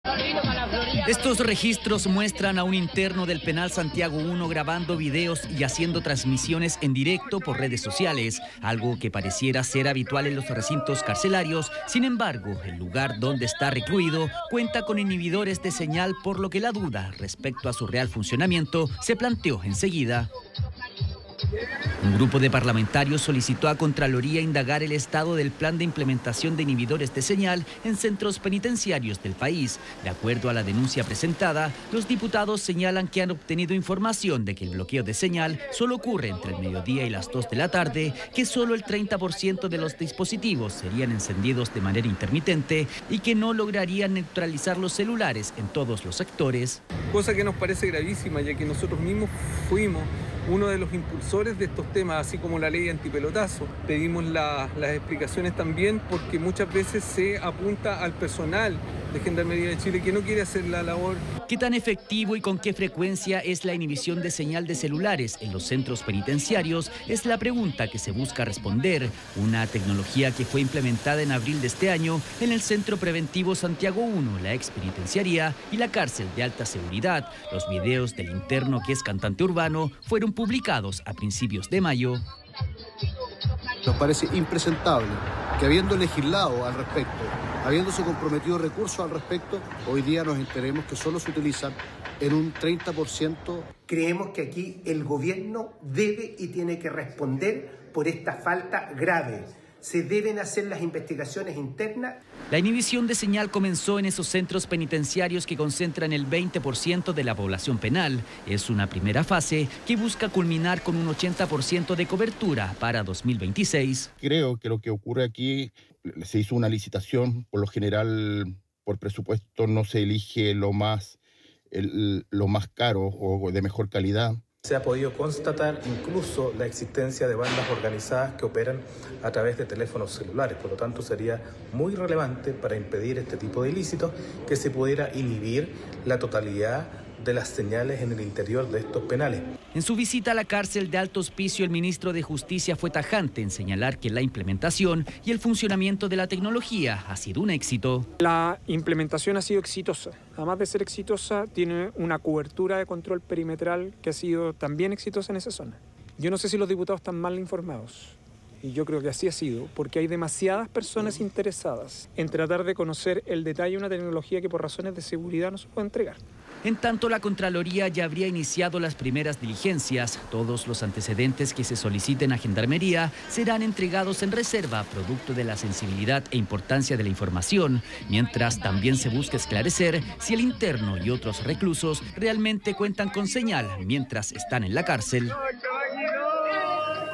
Denuncian uso intermitente de inhibidores de señal en recintos penitenciarios
Debido a denuncias de diputados, Contraloría investigará uso de inhibidores de señal en recintos penitenciarios. El grupo de parlamentarios asegura que la tecnología se utiliza de manera intermitente.